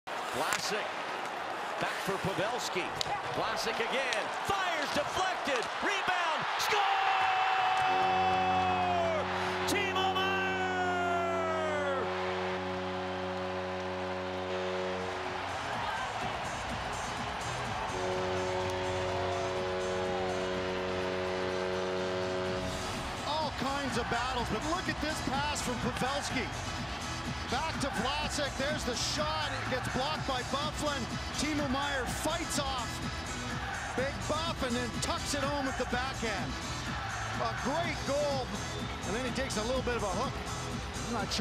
Timo Meier Goal vs WPG 01-23-18
Timo Meier 11 goal of 2017-18 nhl season. even goal assisted by Marc-Edouard Vlasic and Joe Pavelski